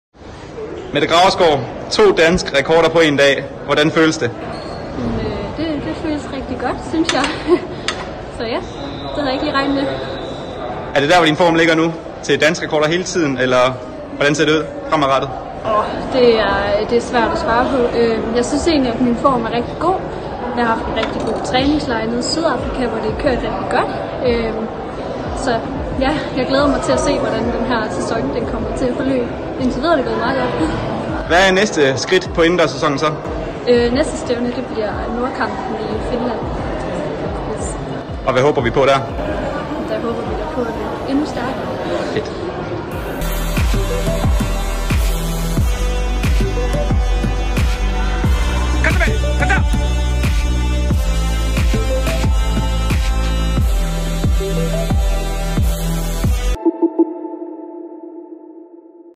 Mette Graversgaard efter to danske rekorder i to forskellige discipliner
Mette Graversgaard har gjort den 1. februar 2020 til en historisk dag for den danske atletik. Hun slog i Wien to danske rekorder. Først på 60m Hæk med 8,36 sek. og to timer efter på 200m med 23,81 sek.

“Mette er i kanongod form i øjeblikket. Fysisk har hun aldrig været bedre, og nogle tekniske ændringer vi har implementeret i løbet til første hæk bærer nu for alvor frugt. Jeg vil ikke blive overrasket om der skulle komme yderligere forbedringer de kommende uger.”, skrev Mikkel Larsen til Dansk Atletik kort efter Mette Graversgaard havde sat dansk rekord på 60m Hæk med 8,36 sek. i finalen ved Indoor Track & Field stævnet i Wien i Østrig. I indledende imponerede hun endda også ved at løbe 8,40 sek. Den danske rekord var inden i dag 8,38 sek. af Mathilde U. Kramer. En tid som hun selv tangerede den 28. december 2019. Dengang sagde hun følgende: “Jeg havde nok ikke forventet en sådanne tid, men jeg havde håbet det, da jeg egentlig synes at min træning har kørt godt her henover vinteren. Så jeg er da selvfølgelig glad for lige at kunne sætte denne personlige rekord. Der har altid været et personligt mål at kunne komme derned.”

Nu slår hun altså den danske rekord, og beviser over for sig selv, at hun har nået et meget højt niveau.

SLÅR DANSK REKORD PÅ 200M TO TIMER EFTER 60M HÆK DANSK REKORD
At slå én dansk rekord er imponerende nok. At slå to er endnu mere imponerende. Og at så slå to danske rekorder på to forskellige distancer er næsten utænkeligt. Men lige netop det gjorde Mette Graversgaard, da hun blot to timer efter den første danske rekord slog en ny dansk rekord på 200m med 23,81 sek. Den nu tidligere danske rekord var på 23,87 sek. som Sara Slott Petersen løb i 2016.

“INDTIL VIDERE ER DET GÅET MEGET GODT"
“Jeg synes min form er rigtig god. Jeg glæder mig til at se hvordan den her sæson kommer til at forløbe. Indtil videre er det gået meget godt.”, siger den ydmyge nu dobbelte danske rekordholder i videointerviewet ovenfor, som er taget lige efter den danske rekord på 200m af Simon Hansen. Simon Hansen, der også løb til stævnet og løb 6,89 sek. på 60m. En anden klubkammerat, Martha Rasmussen, var også med i Wien og løb ny PR og ny dansk årsbedste med 56,86 sek. på 400m.

RELEVANTE LINKS
Resultater Indoor Track & Field Vienna